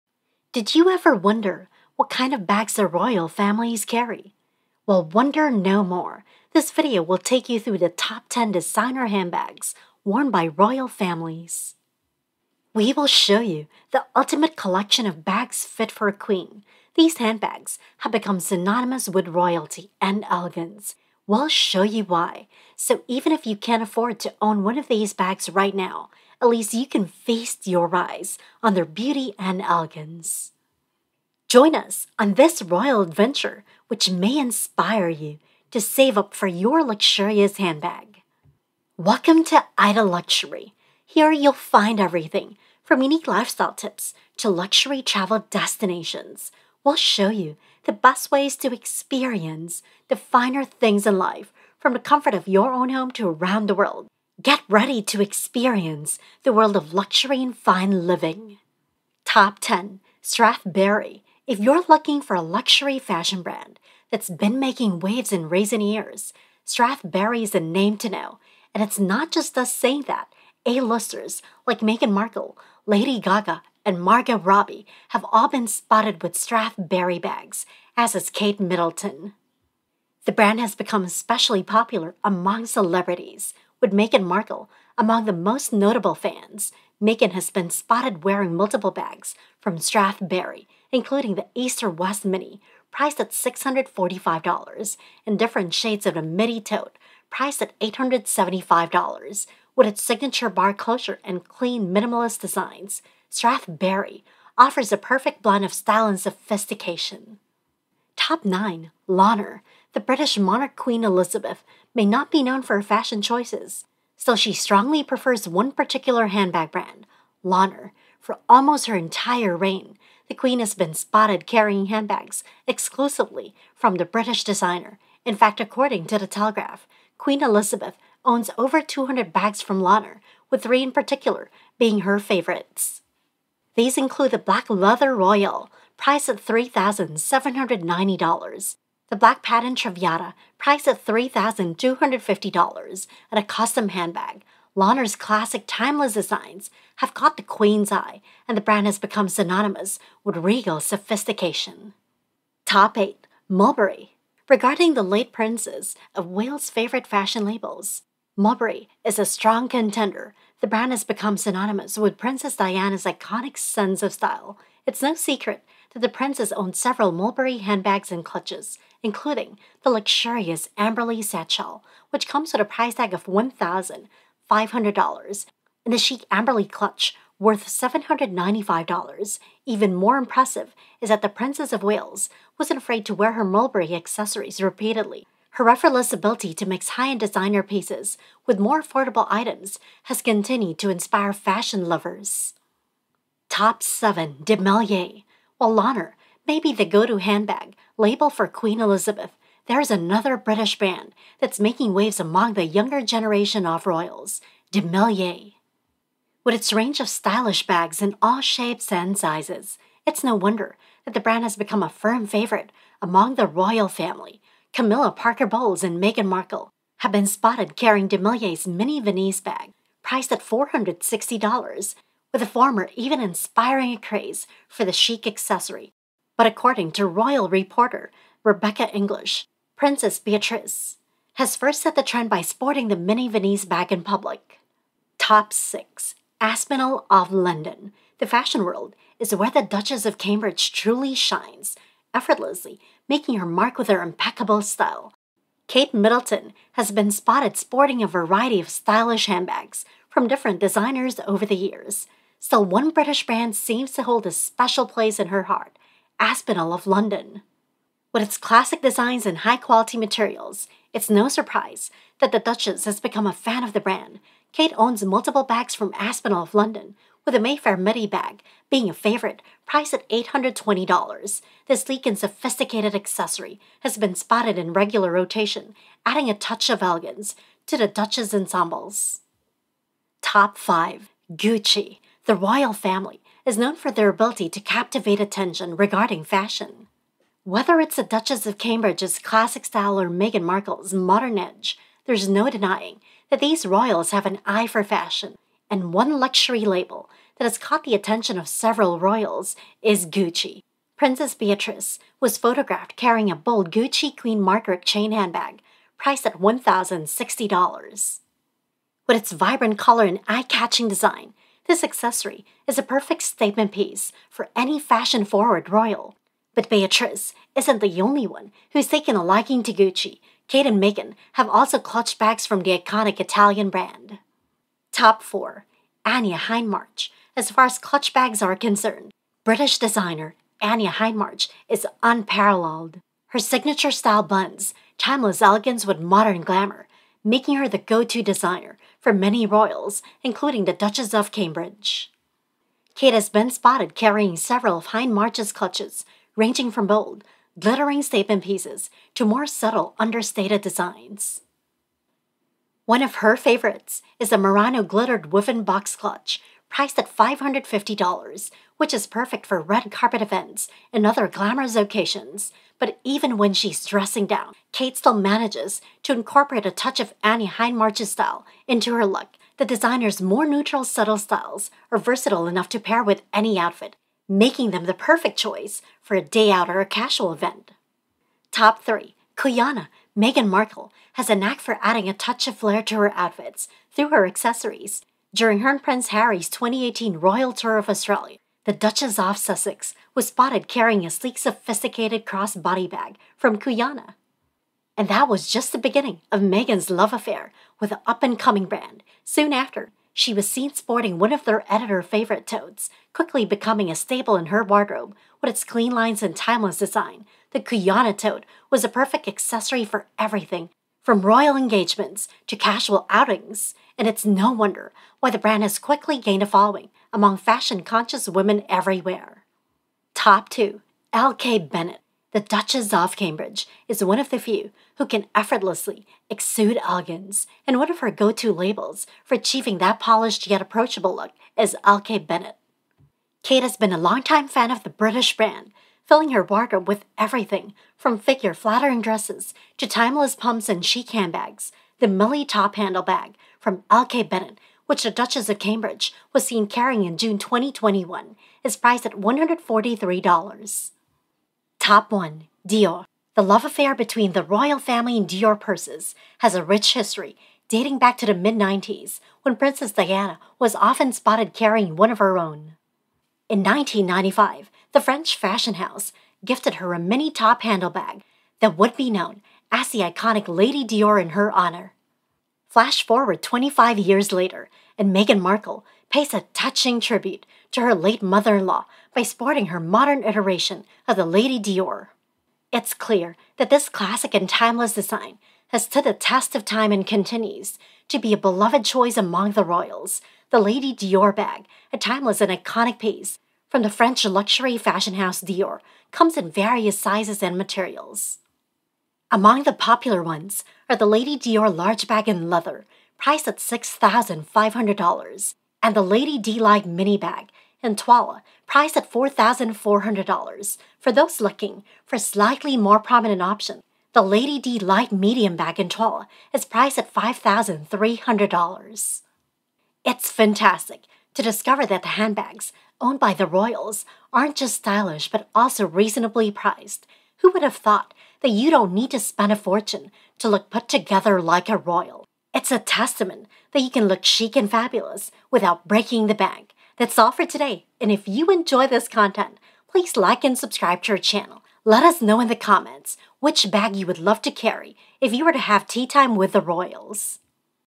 Top 10 Designer Handbags Worn by Royal Families
Welcome to our video listing,  Top 10 Designer Handbags Worn by Royal Families.
         Do you ever wonder what kind of bags the royal families carry? Well, wonder no more! This video will take you through the top 10 Designer Handbags Worn by Royal Families. We will show you the ultimate collection of bags fit for a queen. These handbags have become synonymous with royalty and elegance; we'll show you why. So, even if you can't afford to own one of these bags right now, at least you can feast your eyes on their beauty and elegance. Join us on this royal adventure, which may inspire you to save up for your luxurious handbag.